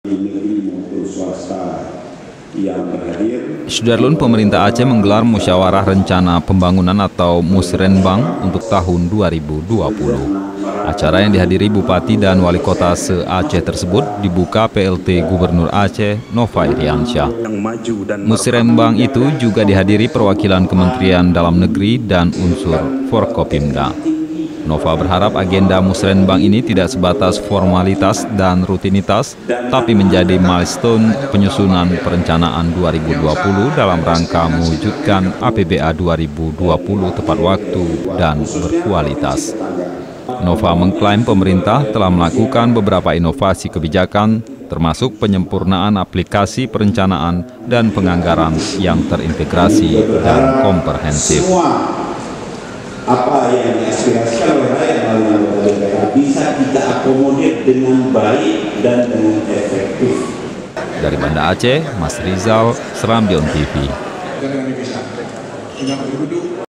Pemerintah Aceh Gelar Musrenbang Tahun 2020. APBA 2020 Diharapkan Tepat Waktu dan Berkualitas.
SERAMBITV.COM, BANDA ACEH - Pemerintah Aceh menggelar musyawarah rencana pembangunan (musrenbang) untuk tahun 2020, Rabu (10/4/2019).Acara yang dihadiri Bupati dan Wali Kota se Aceh tersebut dibuka oleh Plt Gubernur Aceh, Nova Iriansyah.Musrenbang itu juga dihadiri perwakilan Kementerian Dalam Negeri dan unsur forkopimda.Nova berharap agenda musrenbang ini tidak sebatas formalitas dan rutinitas, tapi menjadi milestone penyusunan perencanaan 2020 dalam rangka mewujudkan APBA 2020 tepat waktu dan berkualitas.Nova mengklaim pemerintah telah melakukan beberapa inovasi kebijakan, termasuk penyempurnaan aplikasi perencanaan dan penganggaran yang terintegrasi dan komprehensif.